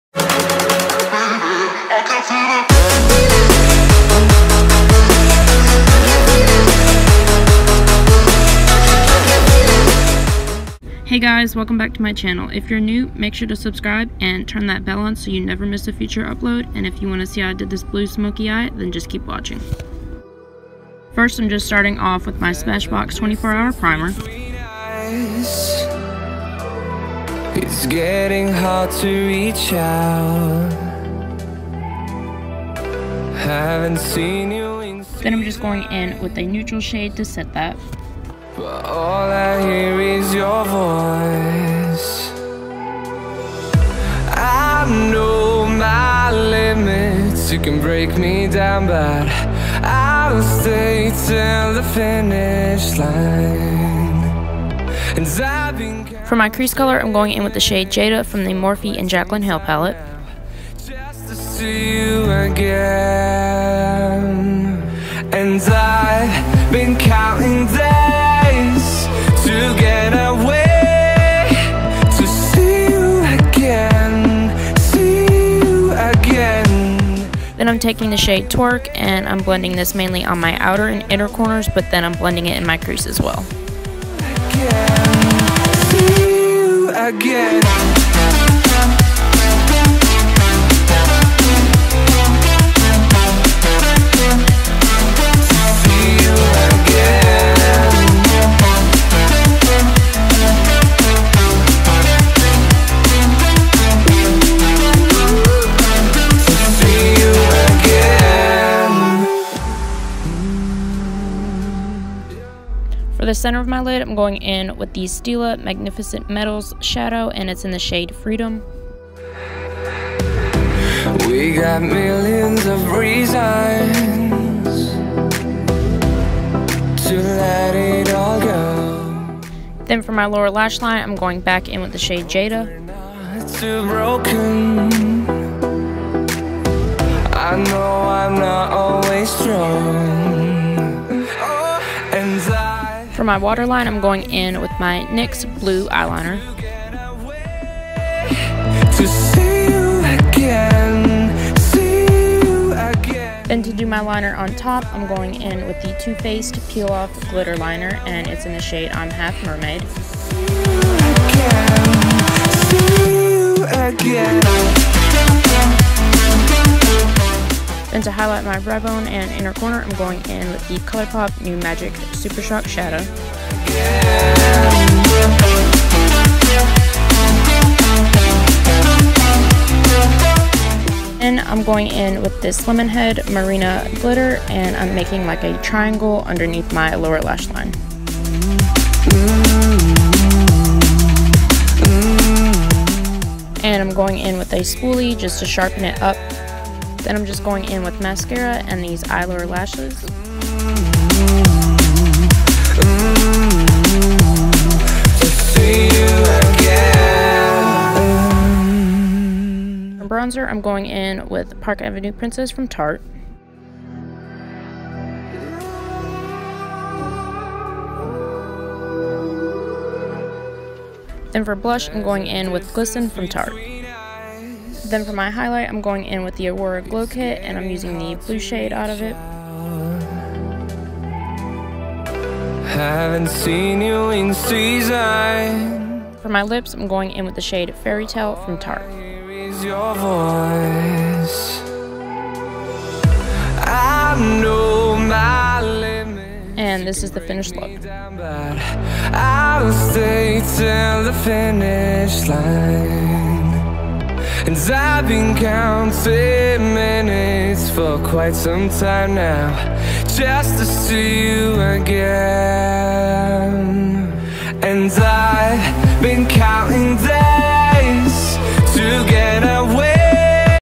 Winter Wonderland Glam | Blue Smokey Eye
Hey guys today’s video is a winter wonderland Blue Smokey Eye look using the morphexjaclynhill palette with a bright teal lip from tarte cosmetics. ❄️❄️❄️